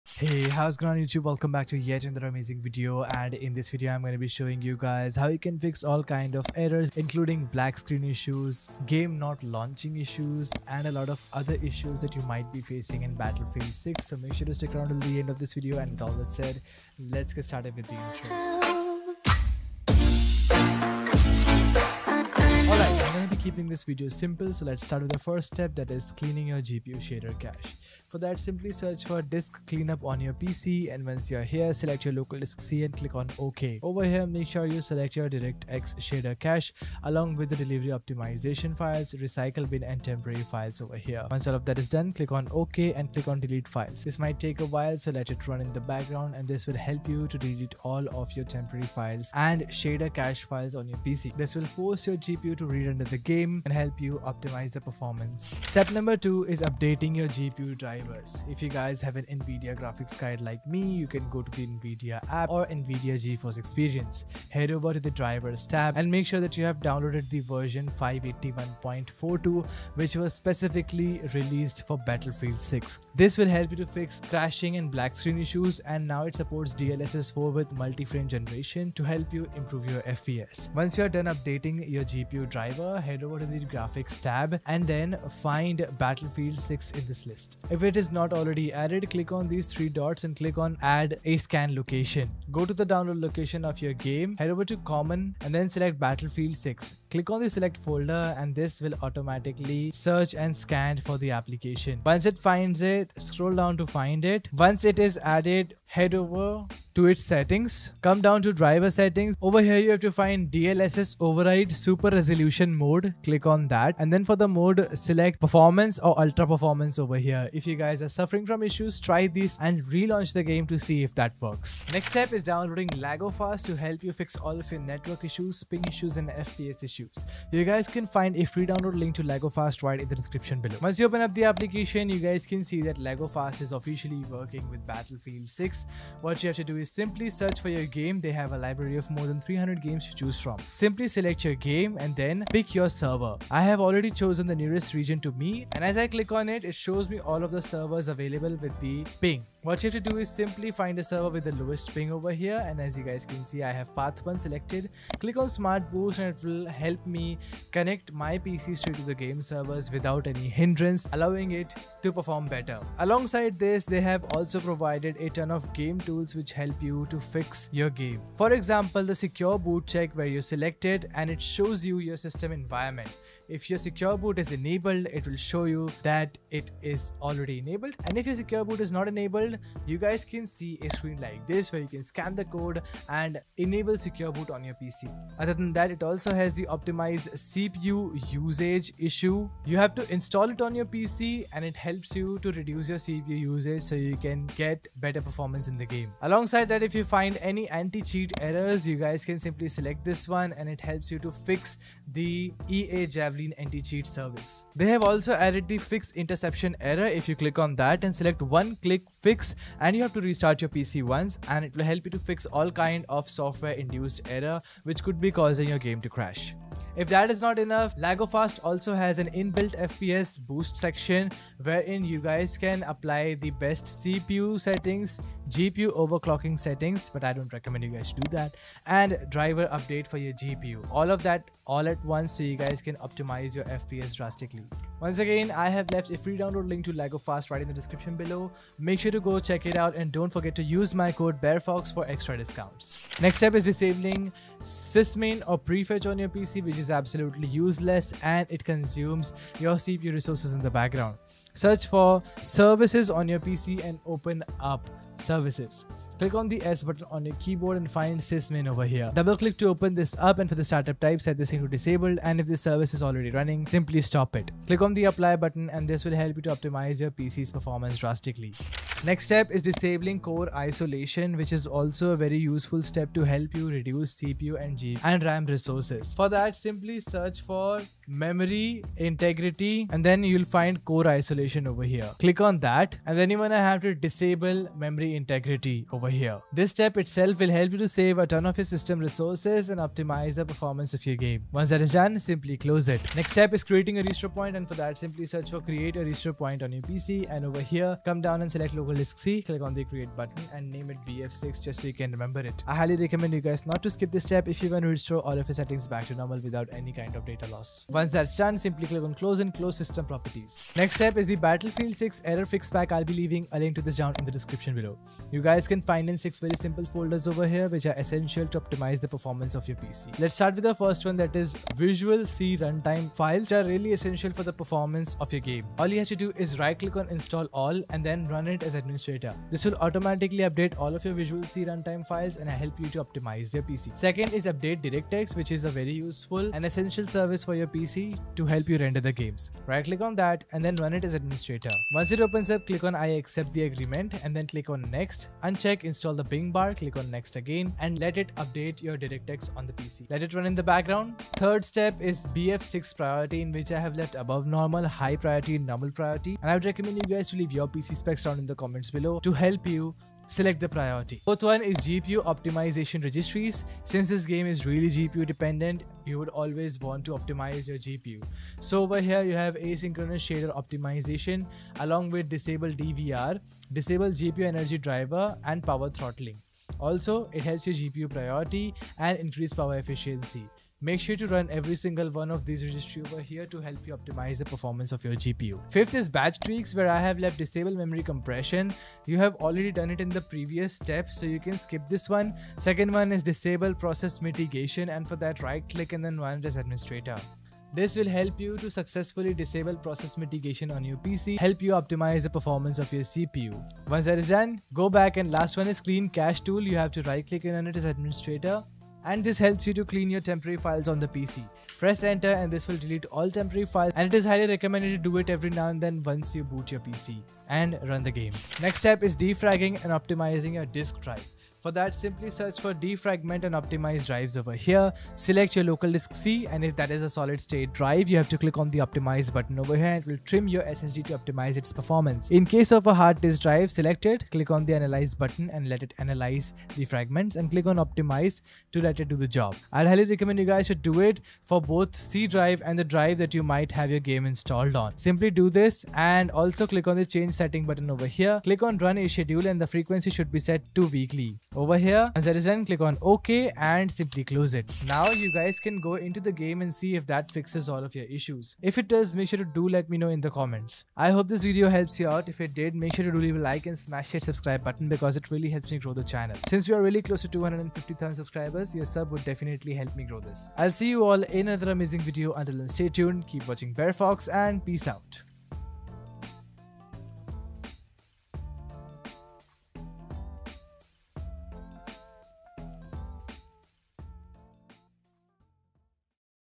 How to Fix Battlefield 6 Errors - Fix Crashing, Insufficient VRAM Requirement & Black Screen Errors
[USE CODE: BareFox]
💥 Facing crashes, VRAM errors, or black screens in Battlefield 6? Don’t worry — in this video, BareFox breaks down the most effective fixes for every major Battlefield 6 issue in 2025 so you can get back to smooth gameplay fast!

🔽 Download Links 🔽

▬▬▬▬▬▬▬▬▬ஜ۩۞۩ஜ▬▬▬▬▬▬▬▬▬
▬▬▬▬▬▬▬▬▬ஜ۩۞۩ஜ▬▬▬▬▬▬▬▬▬

🔽 Timestamps 🔽
0:00 - Amazing Intro
0:22 - Clean GPU Shader Cache
0:55 - Update GPU Drivers
2:02 - Fix all Errors in Battlefield 6 using Lagofast
4:03 - Disable Prefetch (Sysmain)
4:29 - Disable Core Isolation
4:53 - Create a Restore Point
5:11 - Install Visual C Runtimes
5:33 - Update DirectX
5:51 - Game Priority Tweaks
6:01 - GPU Optimization Registries
6:25 - Batch Tweaks
6:45 - Clean Cache
6:58 - Defragment & Optimize Drives
7:41 - Amazing Outro